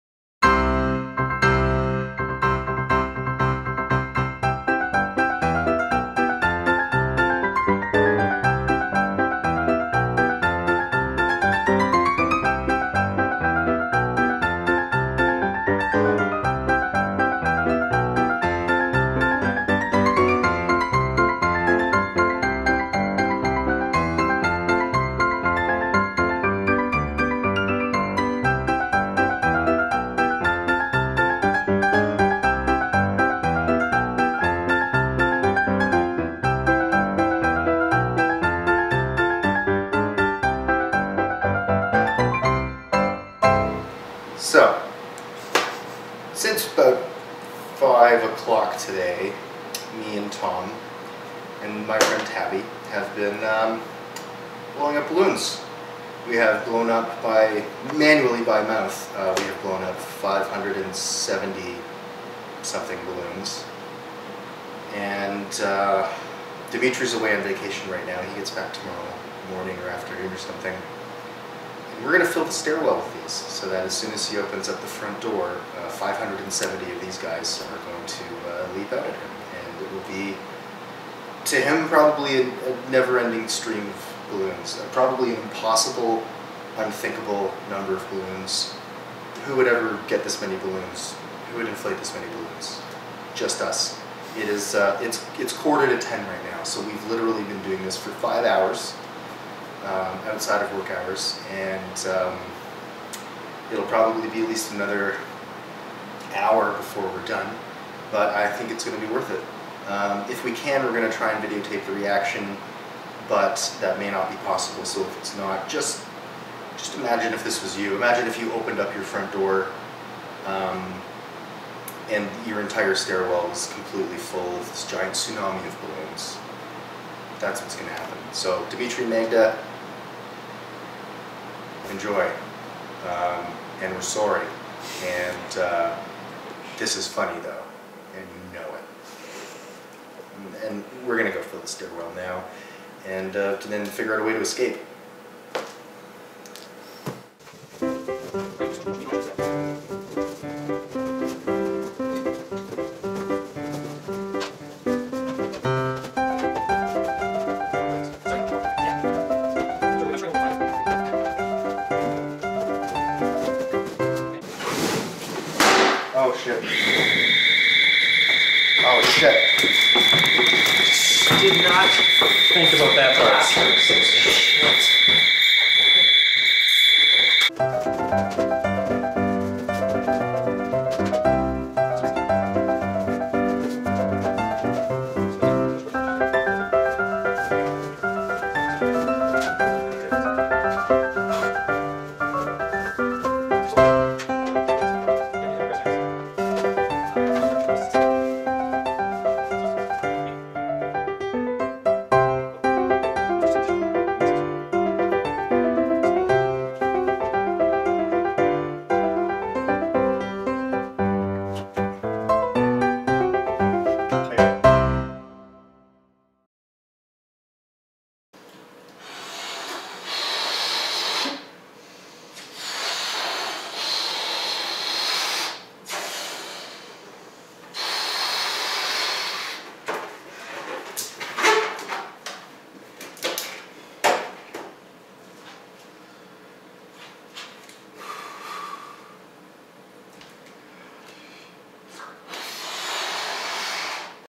Office Prank - 570 Balloons in a Stairwell
Unfortunately, we were not able to record the reaction.